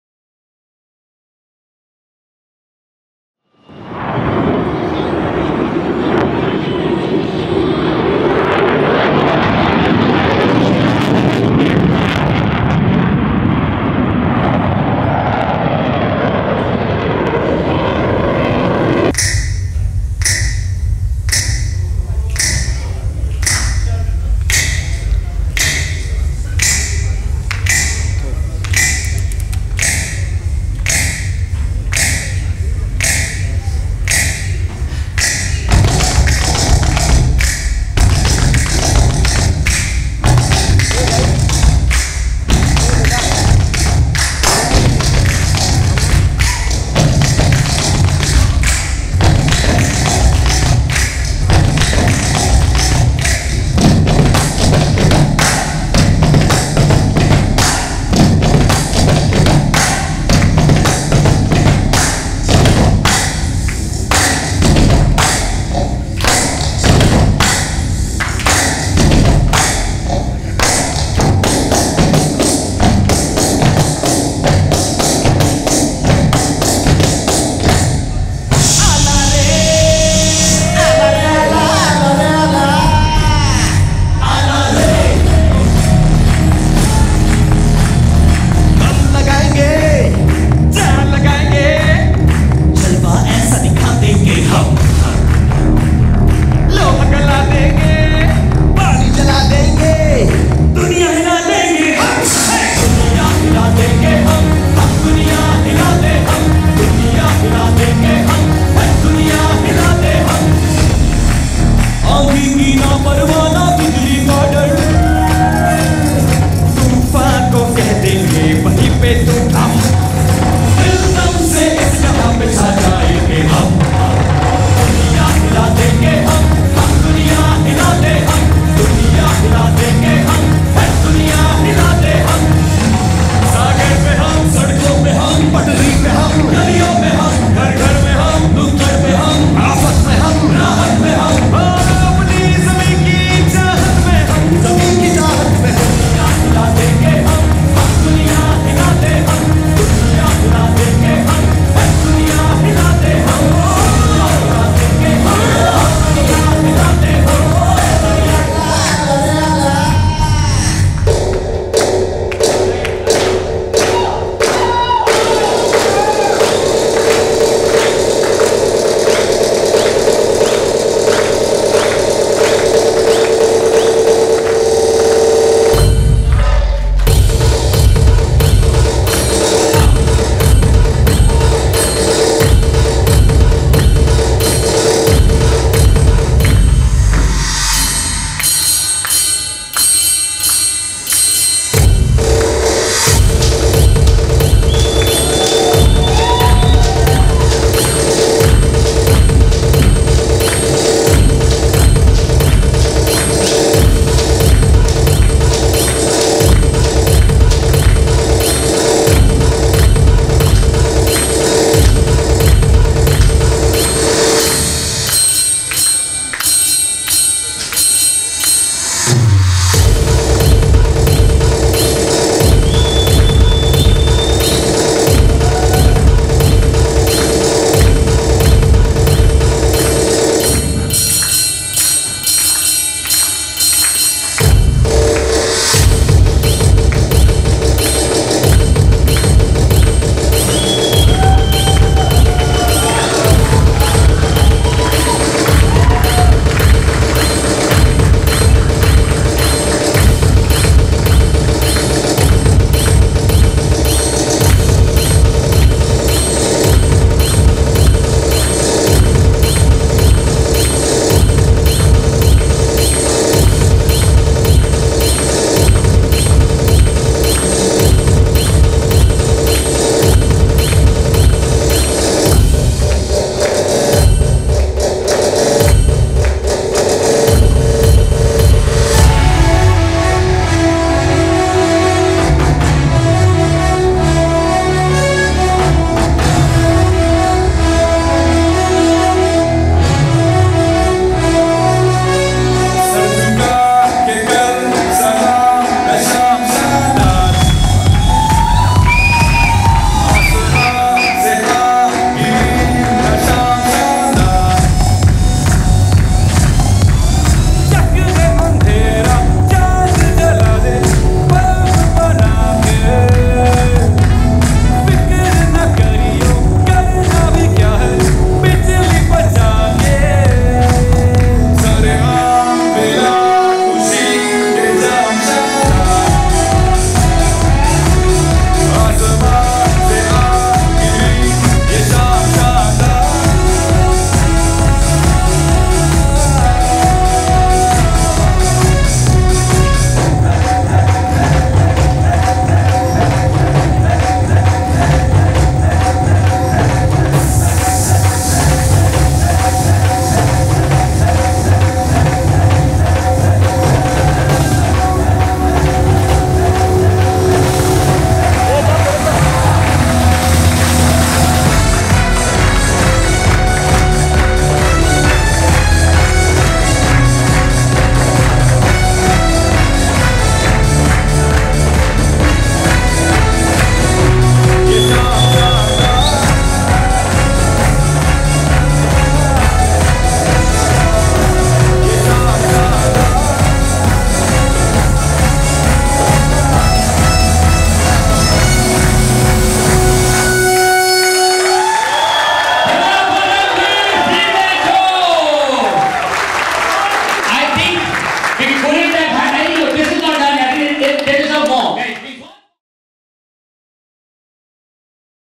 Pidilite Industrial Product Conference Event 2019
This is a conference event held by Pidilite Industrial Product in Radisson Blu Resort Karjat... Choreographer team Kushal Digan
Assistant Dinesh Kamble, Raj Arya.